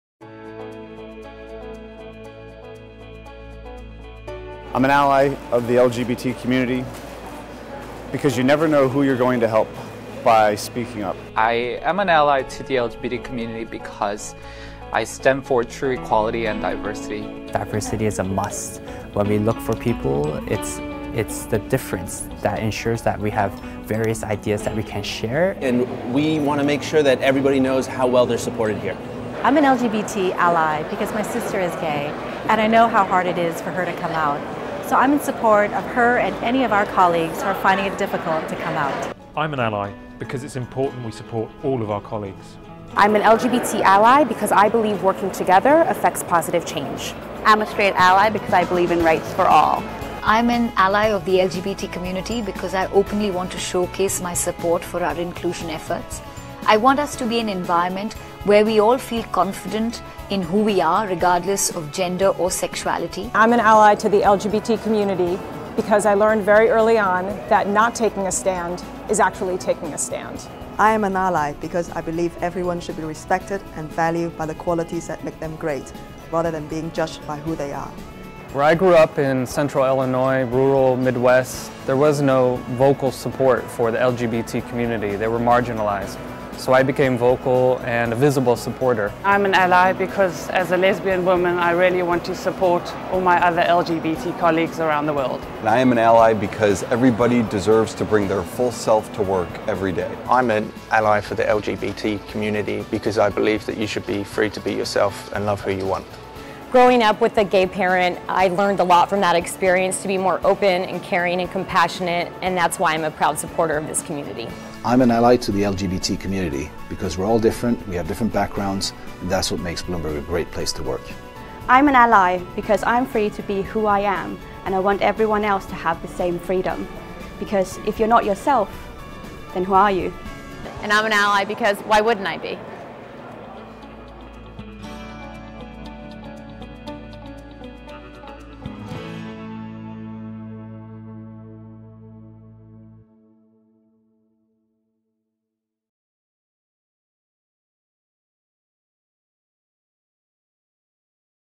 I am an LGBT ally at Bloomberg because ...
Bloomberg employees from around the world share why they are an ally of the LGBT community.